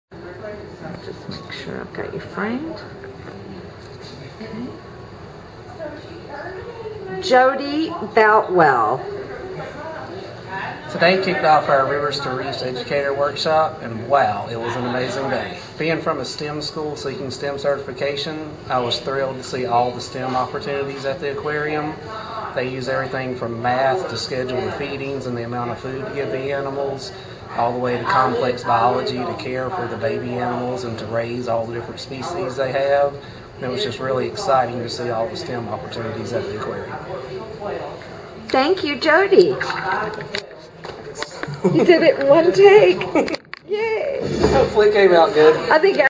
Jody Boutwell - Rivers to Reefs 2016: Day 1
Jody Boutwell talks about the Georgia Aquarium.